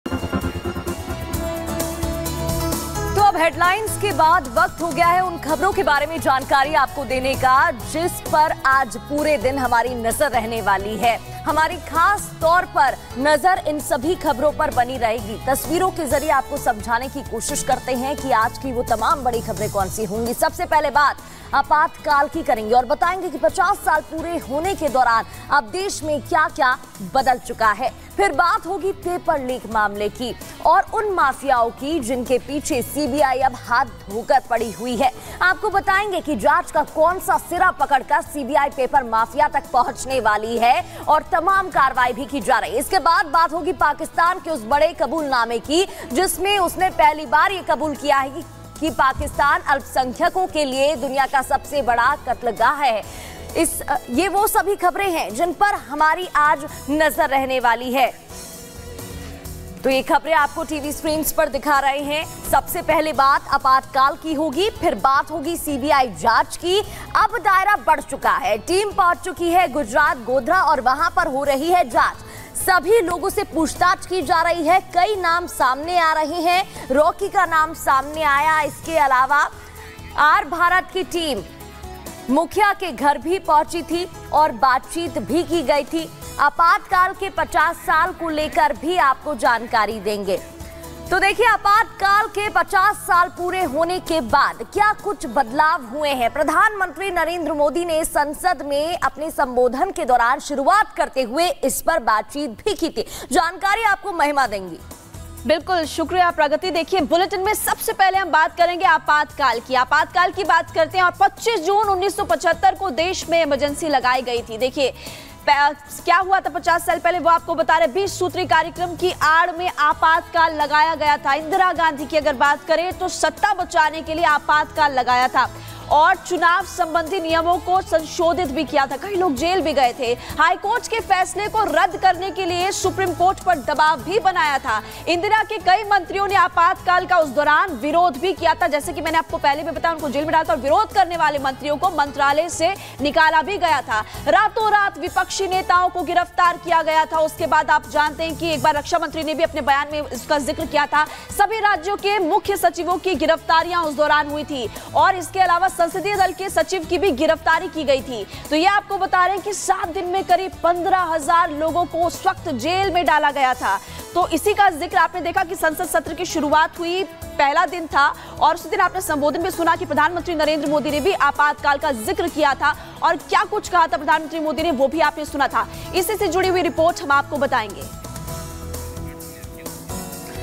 BJP On Emergency: आपातकाल के आज पूरे हुए 50 साल, कांग्रेस के खिलाफ बीजेपी का विरोध प्रदशन |R Bharat
Parliament Session 2024: आज से 18वीं लोकसभा संसद सत्र की शुरूआत हुई. पहले ही दिन संसद भवन में जमकर हंगामा हुआ. पीएम मोदी ने अपने भाषण में आपातकाल का जिक्र किया, विपक्ष ने संविधान की कॉपी हाथ में लेकर विरोध किया, और क्या-क्या हुआ जानने के लिए देखें वीडियो.